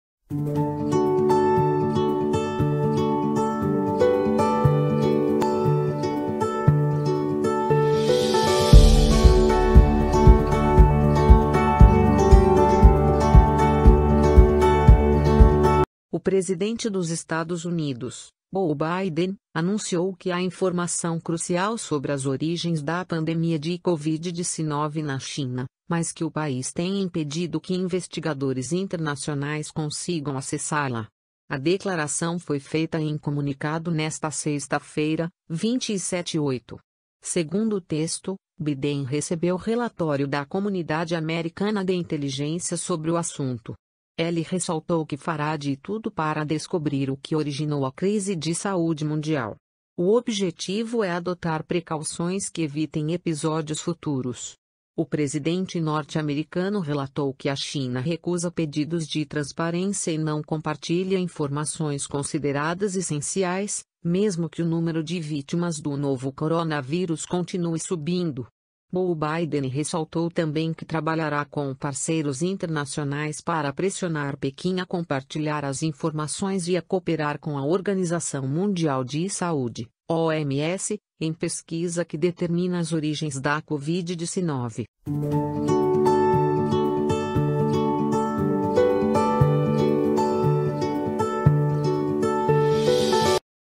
Biden acusa China de reter “informação crucial” sobre origem da Covid
O presidente dos Estados Unidos, Joe Biden, anunciou que há “informação crucial” sobre as origens da pandemia de Covid-19 na China, mas que o país tem impedido que investigadores internacionais consigam acessá-la.